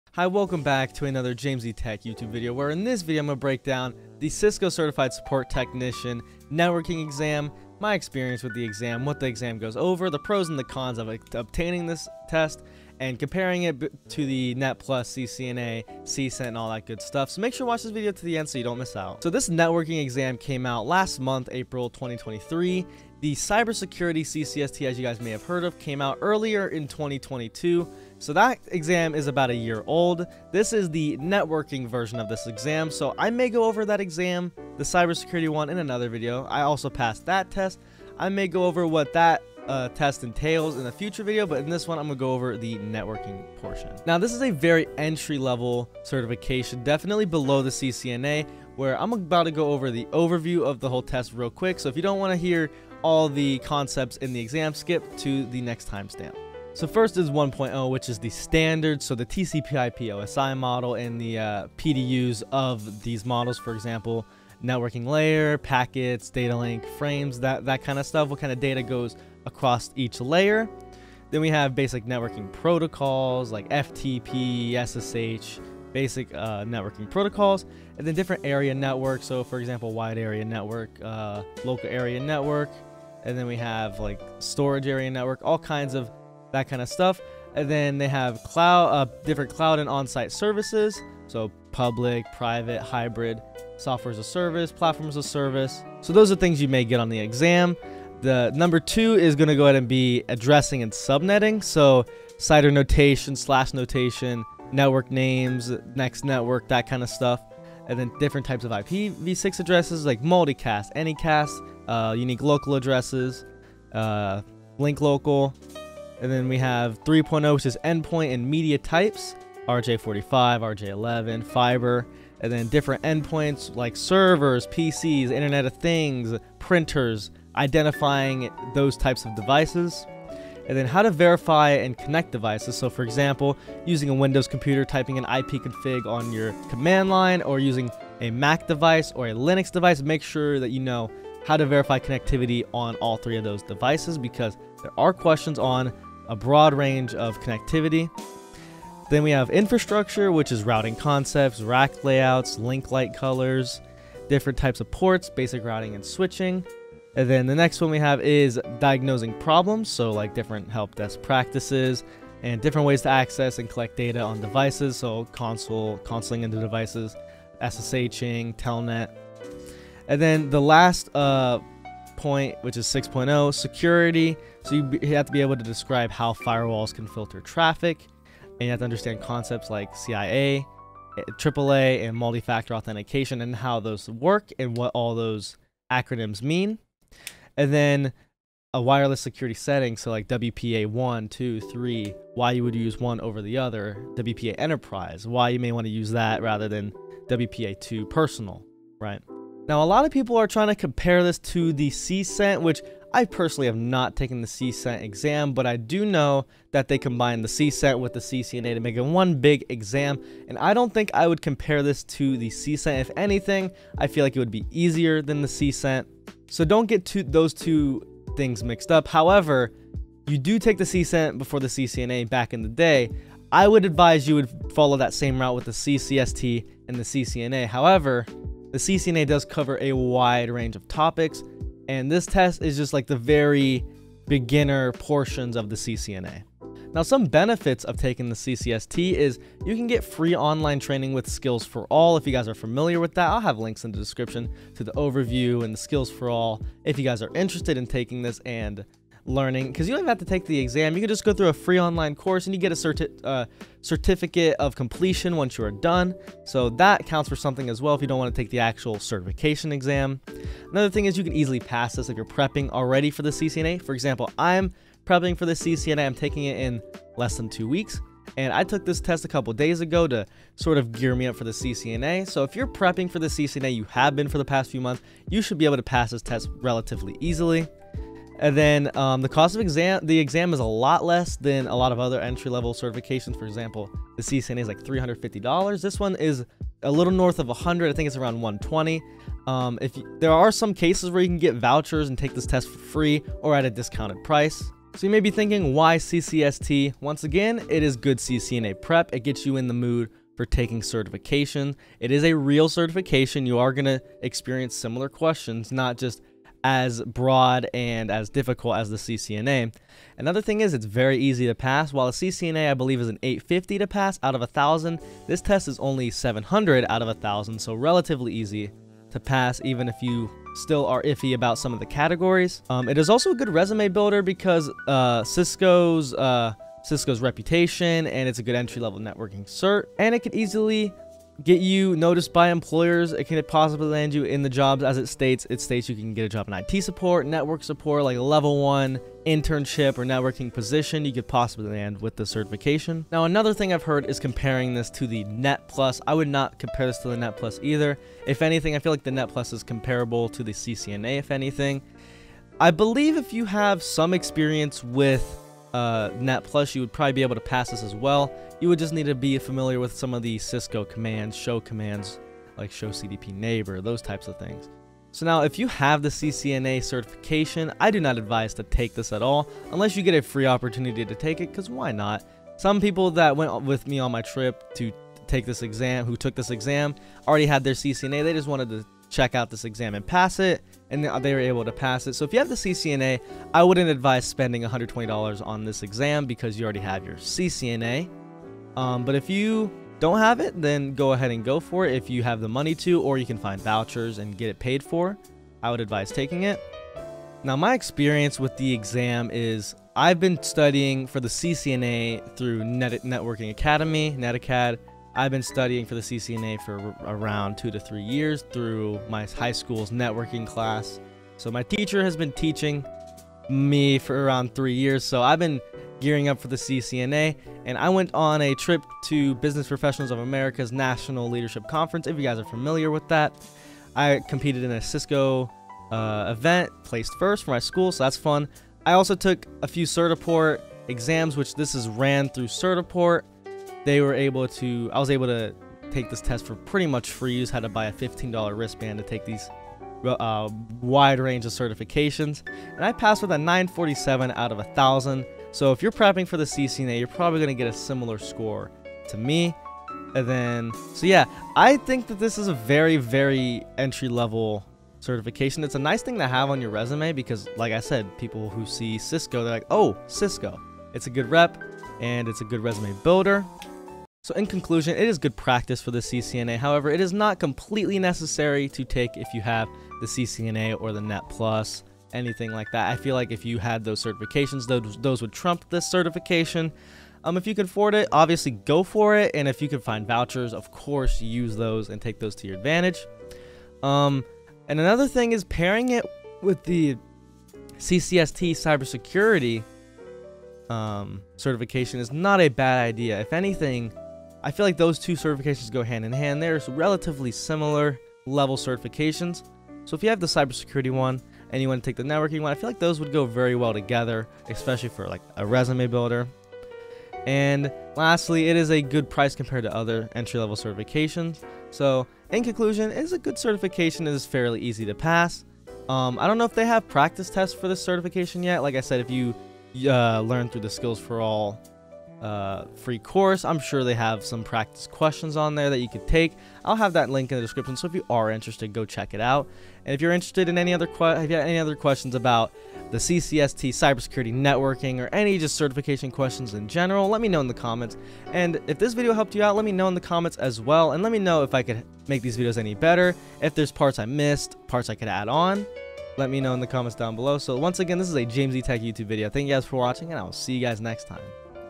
Cisco Certified Support Technician Networking | New Entry-Level Networking Certification! | CCST
In this video, I will review the new CCST (Cisco Certified Support Technician) certification in Networking. This certification was just released and in this video, I talk about the objectives, compare it to other certifications, show the benefits and disadvantages, and talk about my experience taking the exam!

Intro: 0:00
What is the CCST: 0:44
CCST VS. CCENT: 3:19
Benefits of the CCST: 3:57
Why CCST?: 4:55
CCST With CCNA?: 6:04
My Experience with CCST: 6:37
Conclusion: 8:31